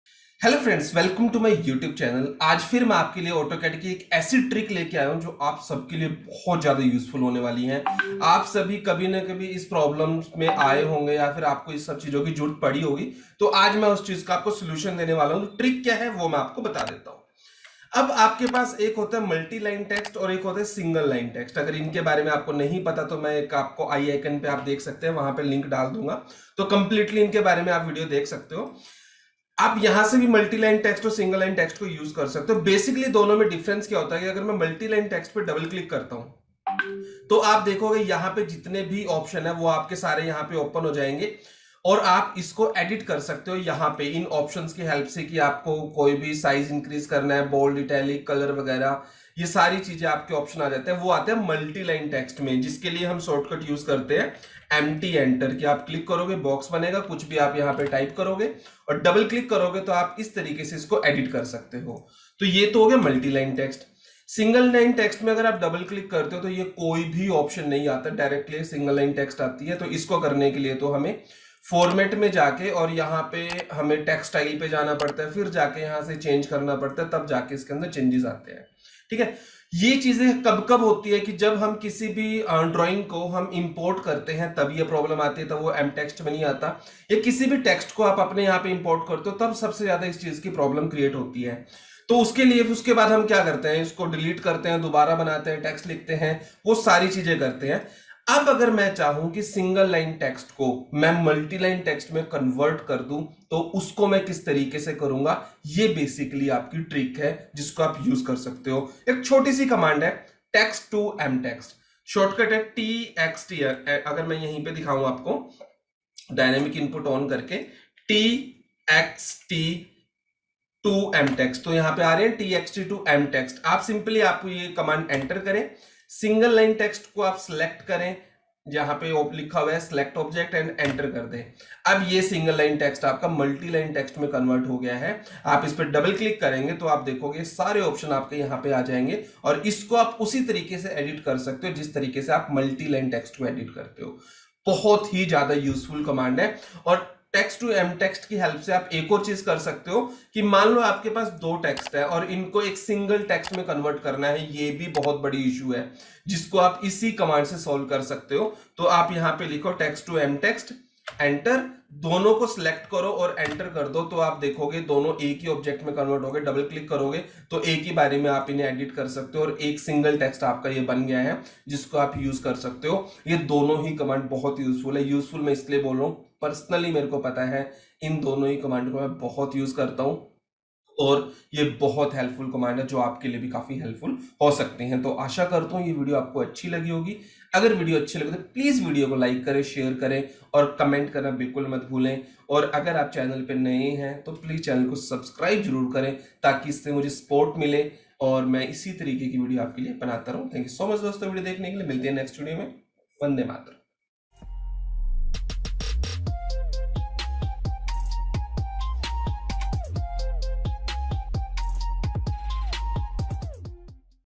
AutoCAD Tricks || Convert Text to MText || SingleLine Text to MultiLine Text [AutoCAD Hacks]
Learn how to convert Single Line Text (TEXT) into Multi-Line Text (MTEXT) in AutoCAD with a simple command. This trick improves text editing, formatting, and organization in your drawings. Watch the video to streamline your AutoCAD workflow efficiently.